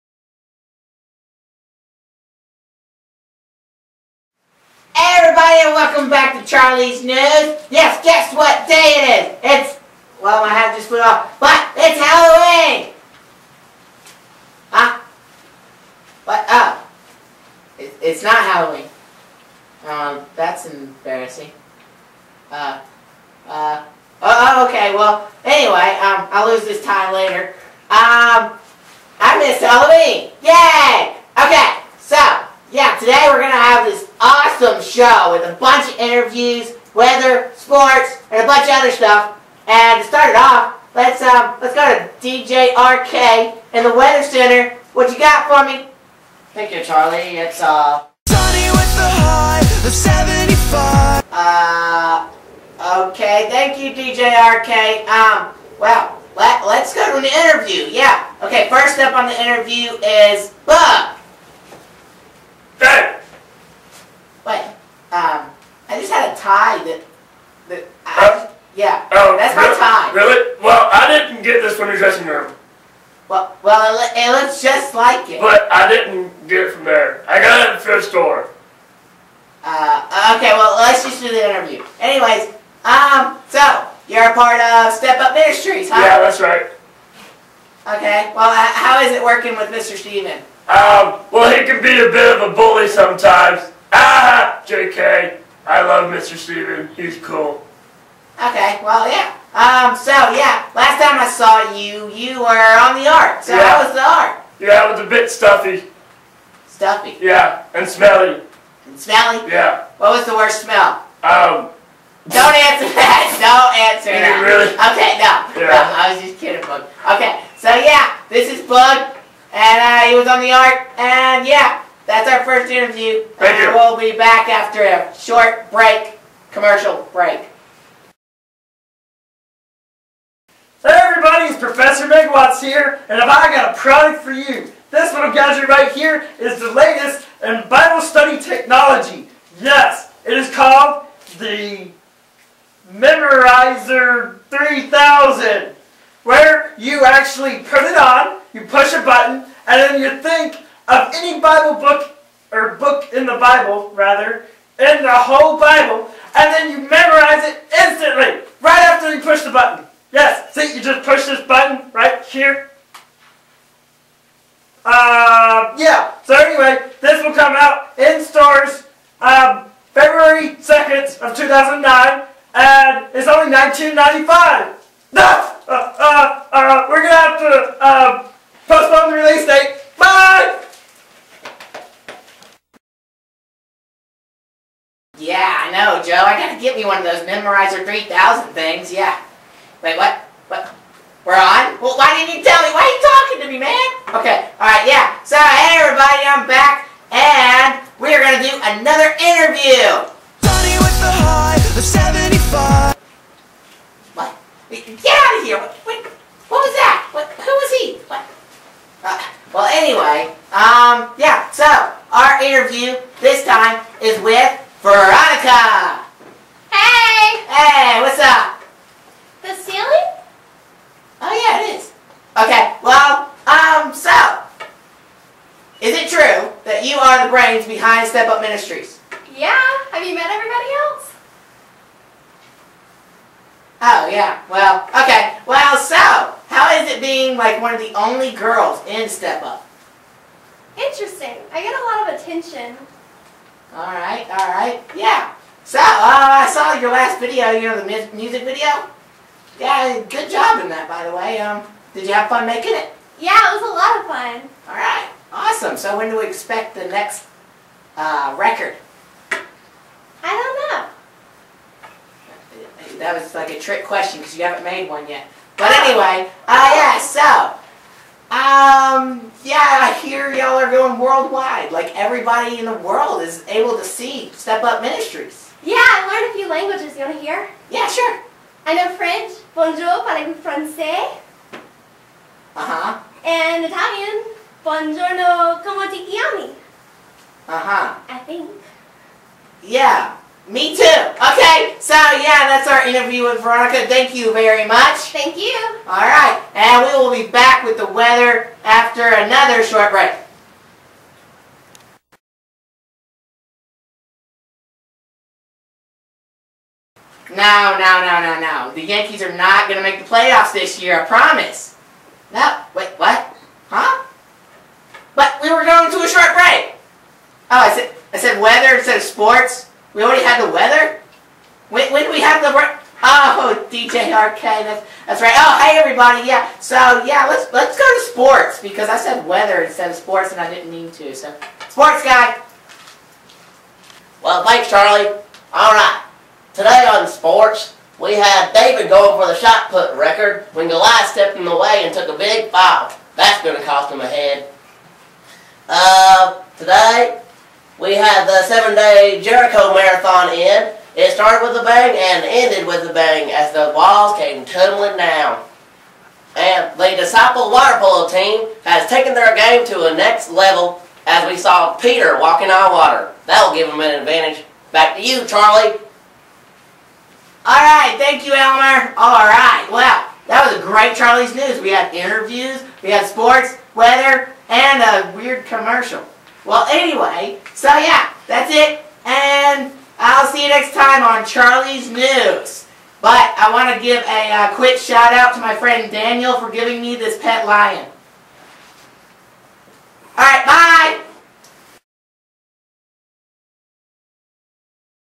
Charlie's News
Steppup Ministries, Charlie of 'Charlie's News' has his day with a full news show from his studio.

steppup ministries charlies news boog veronica puppets video christian childrens ministry step up family  fun anchorman